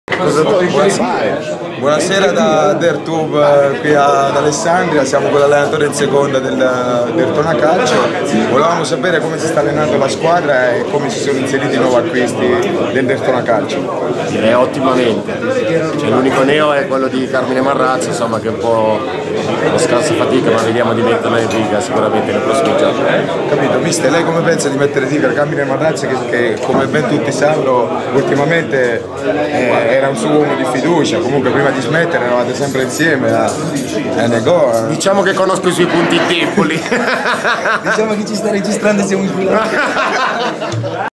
Marrazzo intervista Balsamo
Carmine Marrazzo intervista Mister Lele Balsamo, allenatore in seconda